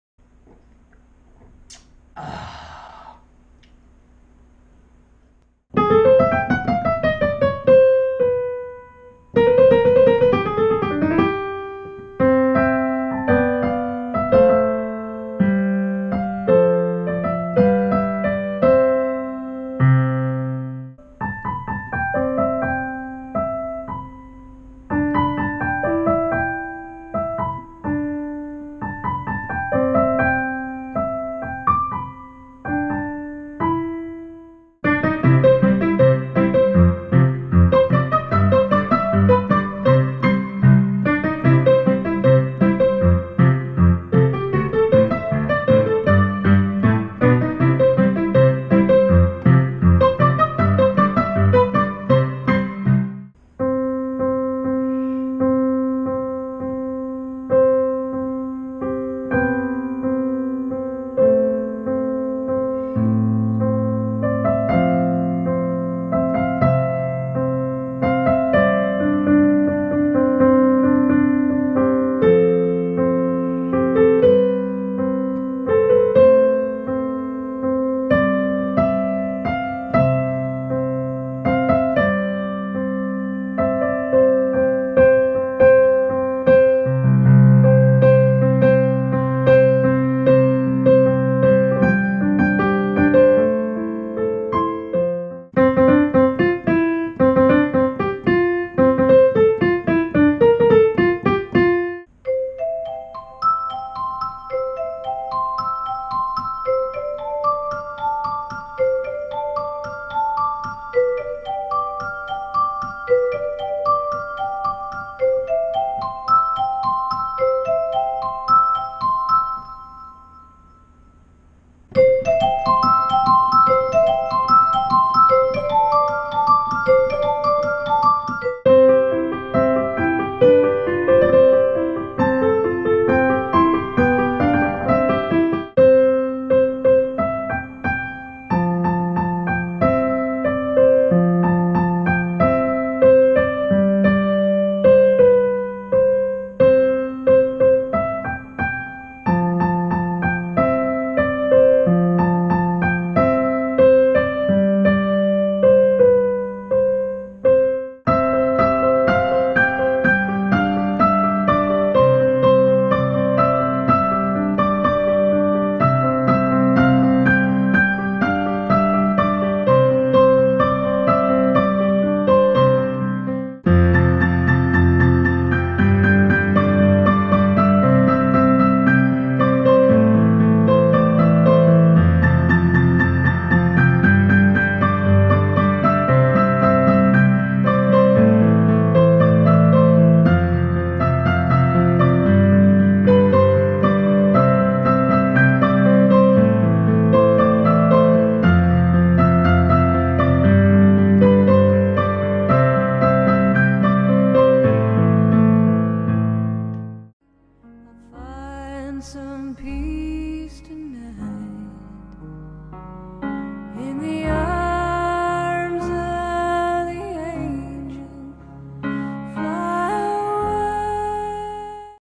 Top 10 Songs Originally Composed in C Major (Played On Piano)
Top 10 Songs Originally Composed in C Major

Hallloooooooo

I am Anthony, I make piano videos and I also make gaming videos every so often.